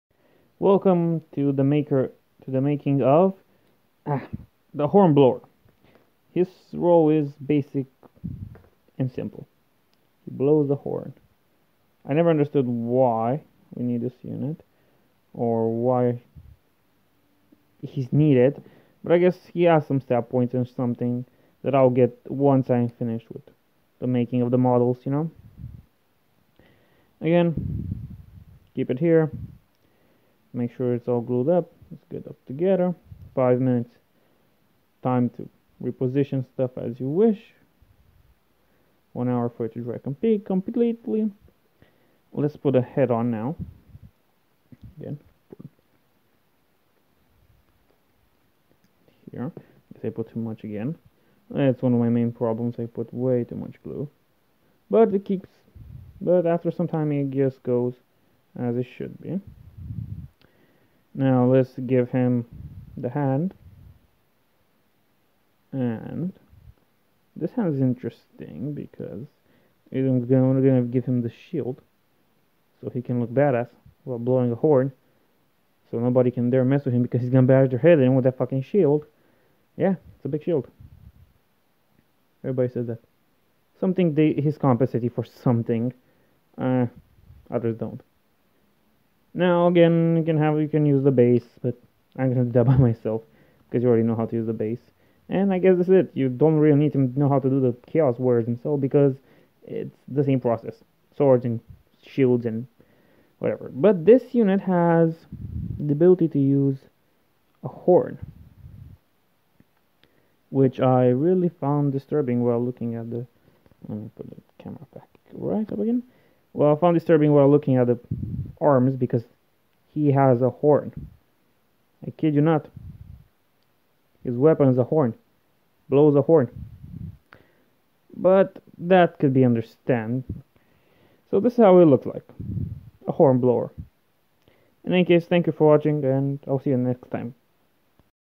Slaves to Darkness Chaos Warriors: Hornblower
When roused for a battle, a Chaos Warrior becomes a roaring, unstoppable force. Arrows and bolts patter from his hell-forged armour like hailstones upon a glacier as he strides into the enemy ranks. The thrusts of spear and halberd are deflected contemptuously, and the lifeblood of his foes spatters his armour as his jagged blade rises and falls.

A numberless horde trampling the realms beneath iron-shod feet, the horn-helmed killers known as the Chaos Warriors are legion. Only Azyr has escaped their wrath - every other mortal realm rings to the sound of their savagery. These men have cast aside everything in favour of eternal war and the promise of power - some may yet bask in the glory they seek, but for most there is only a future of blood, violence and brutal fate.

This kit contains sixteen plastic Chaos Warriors, armed with either Chaos hand weapons and Chaos Runeshields or a Chaos hand weapon in either hand. Supplied with sixteen Citadel 32mm Round bases.